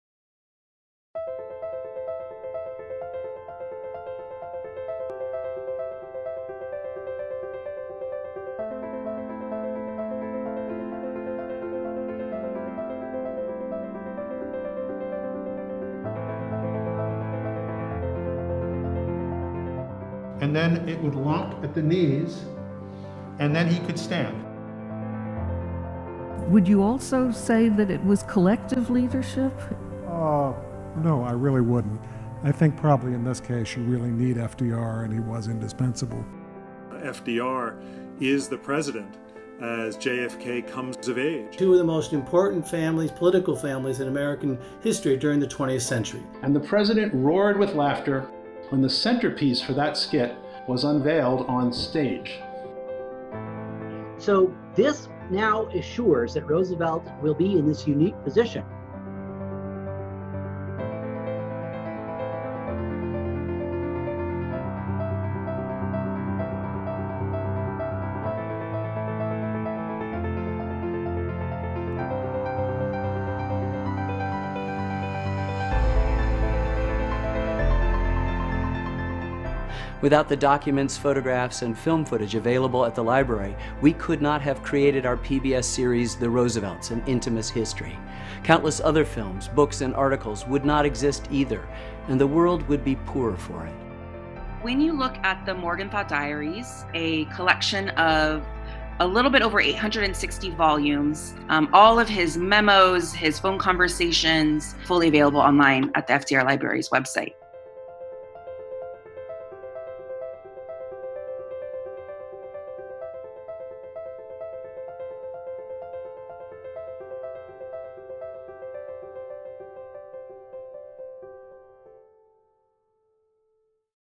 Supporting the Roosevelt Legacy
Your support helps keep the Roosevelt legacy alive through the FDR Presidential Library and Museum’s permanent and special museum exhibits, archive of 17 million documents (whatever that number is), educational engagement, and programs both on site and virtual.